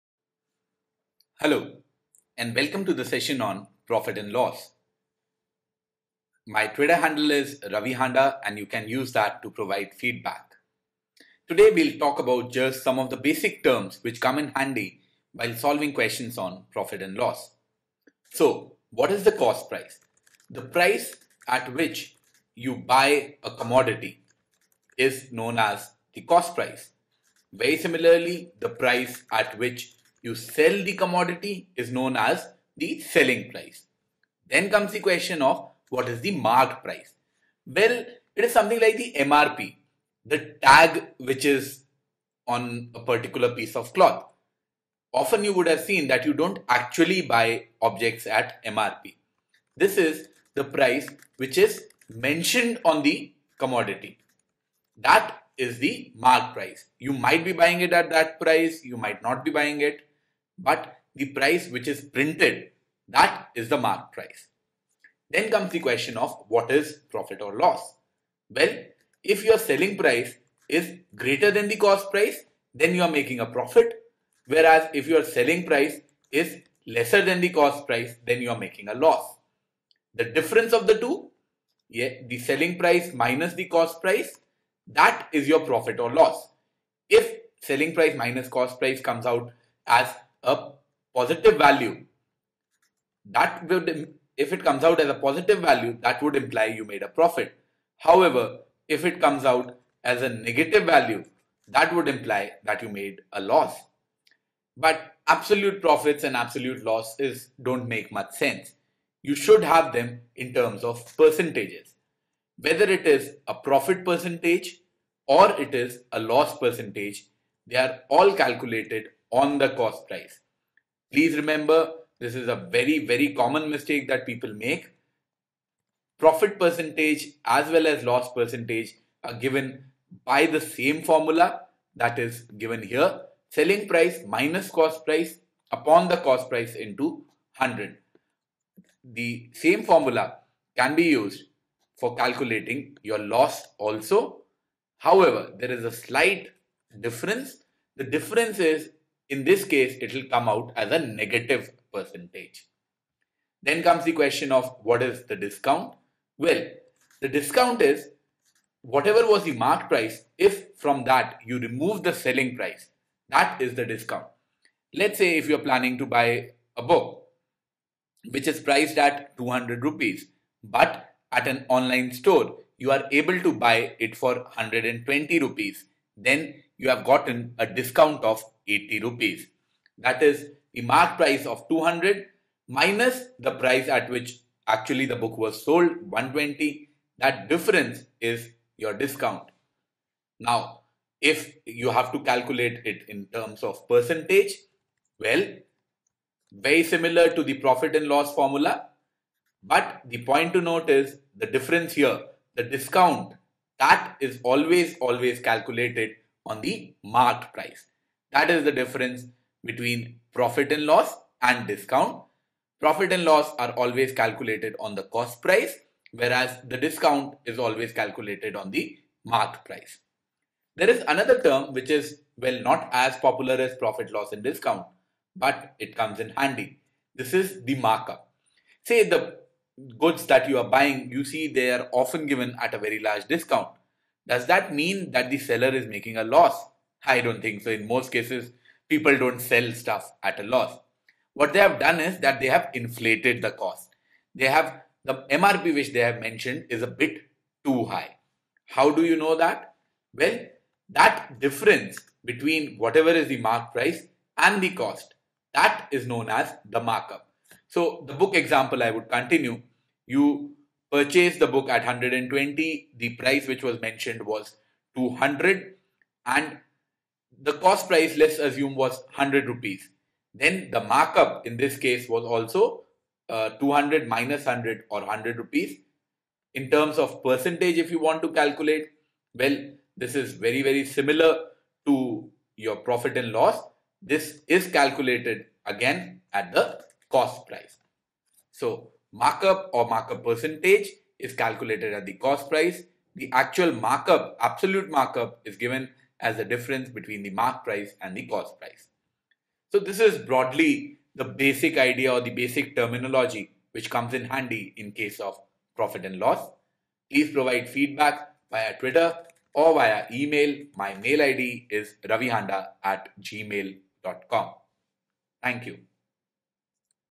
Profit Loss Basic in aptitude Terms New Format
This is the aptitude questions and answers section on "Profit and Loss" with explanation for various interview, competitive examination and entrance test.